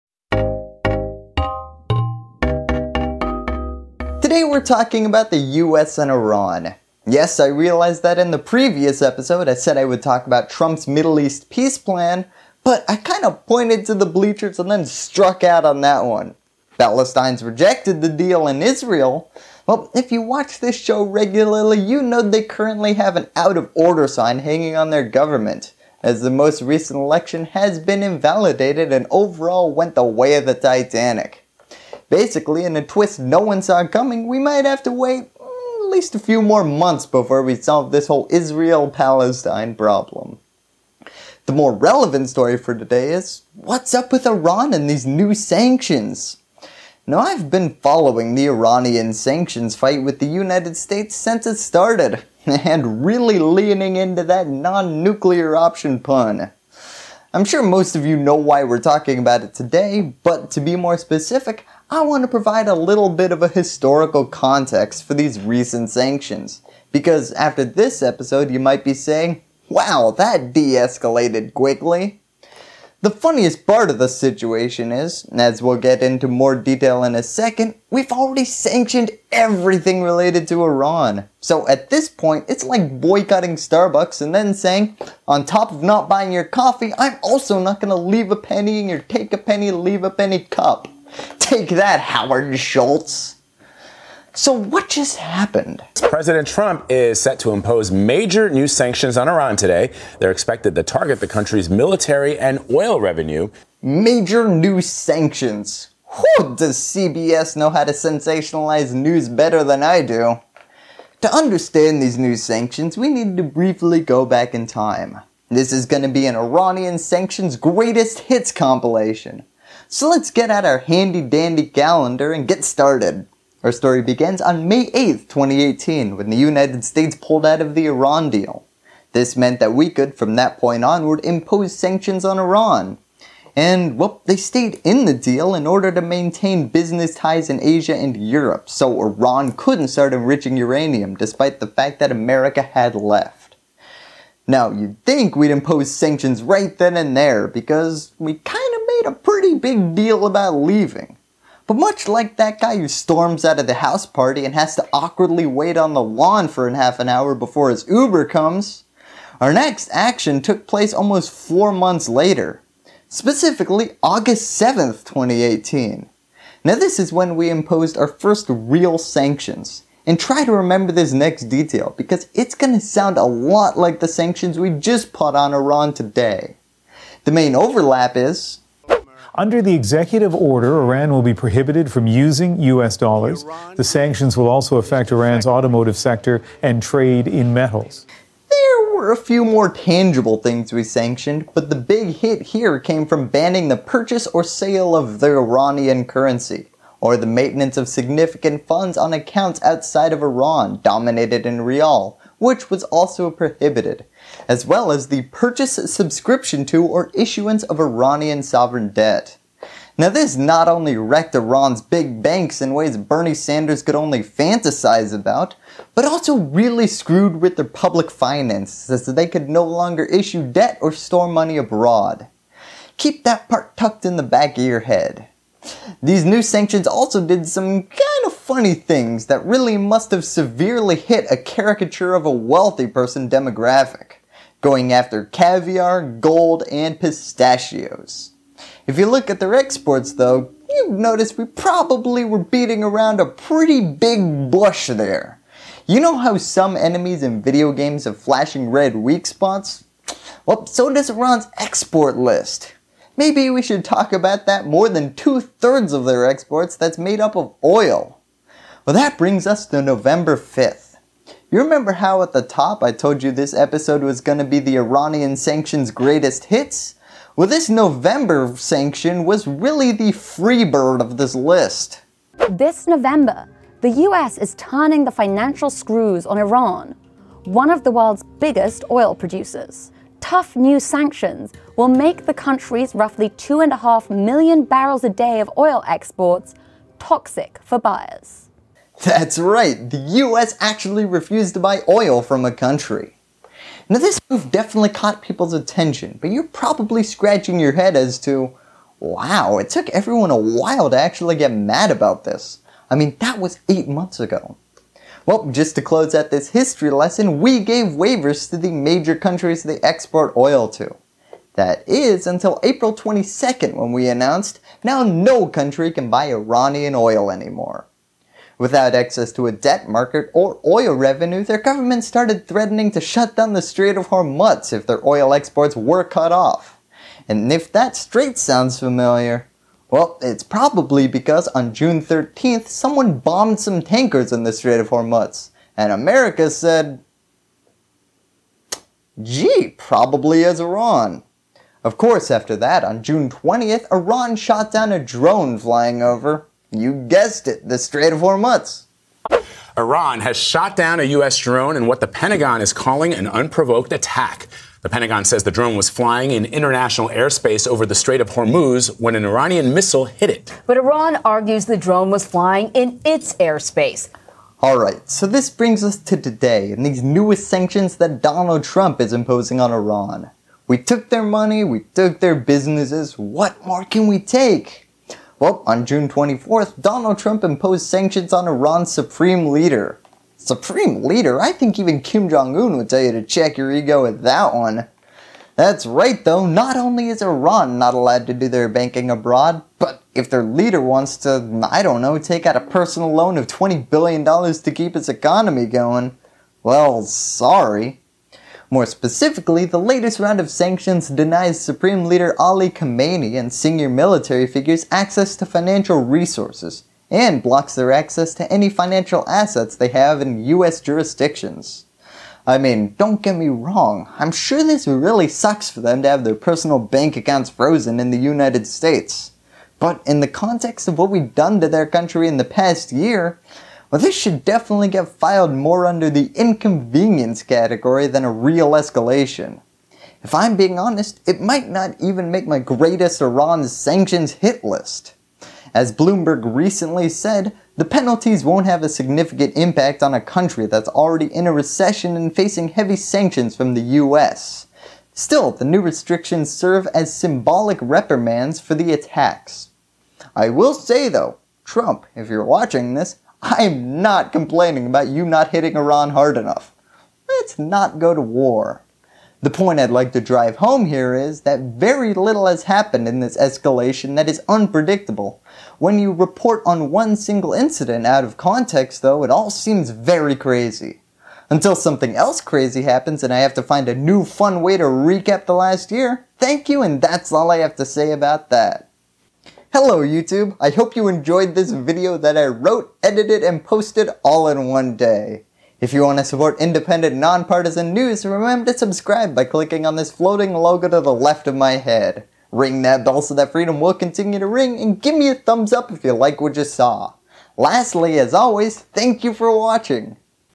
What is Going on With Iran? How We Got to the Brink of War
The strained relationship between Iran and the United States has recently been in the front pages of the news with downed tankers, a downed drone, and the United States canceling attack plans at the last moment. How, over the last two years did our relationship go from an Iran Deal to the brink of war. One word; Sanctions.